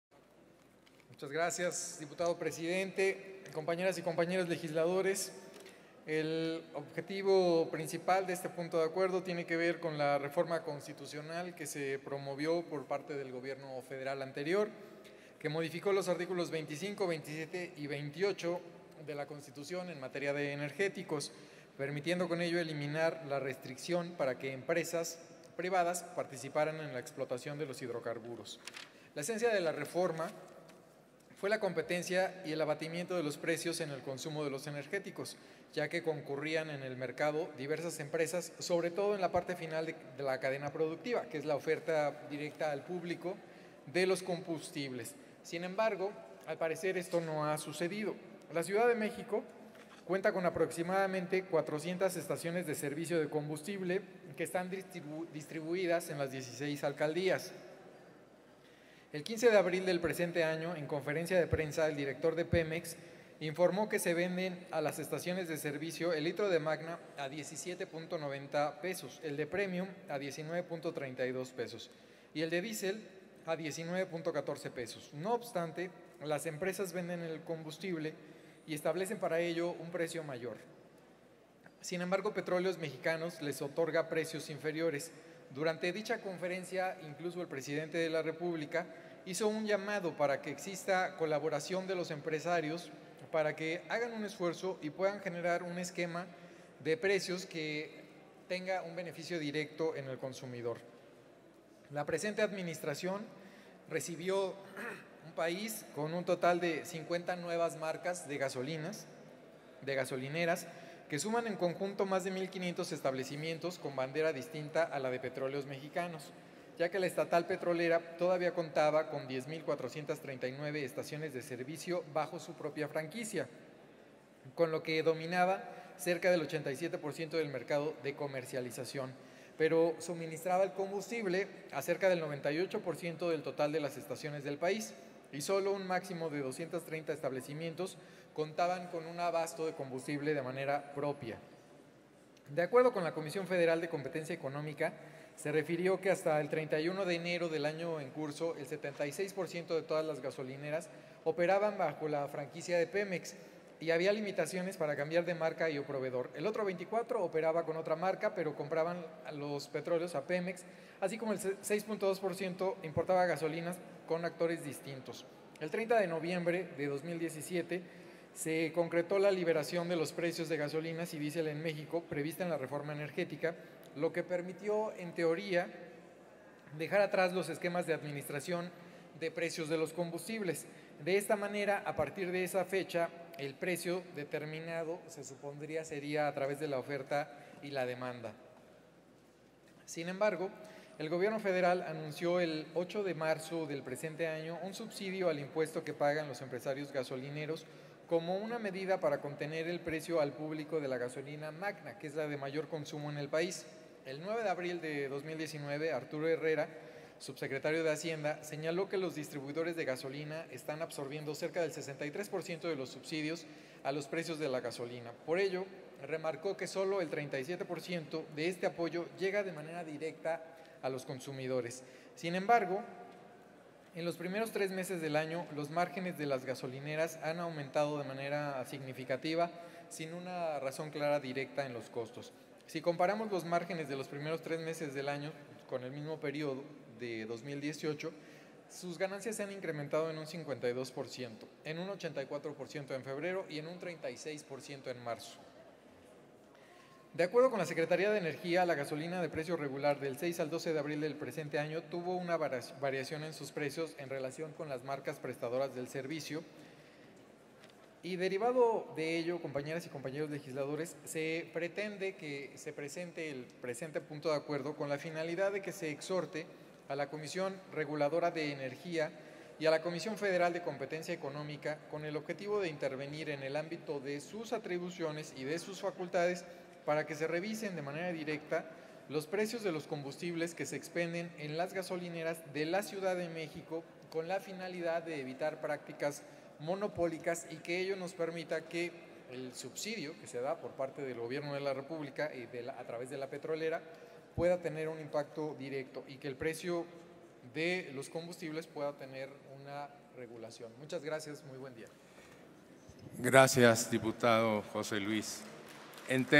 25-04-2019 Punto de acuerdo
Hoy presente un Punto de acuerdo en donde se exhorta a la Comisión Reguladora de Energía y a la Comisión Federal de Competencia Económica a revisar los precios del combustible que expenden las gasolineras en la Ciudad de México.